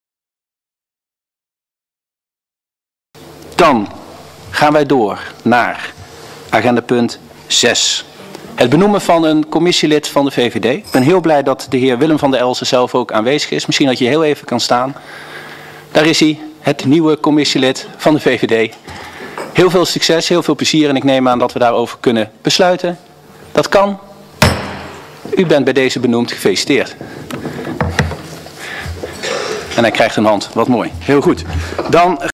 Gemeenteraad 2018 09 26 Punt 6 Benoeming commissielid
Benoeming Commissielid Dhr vd Elsen voor de VVD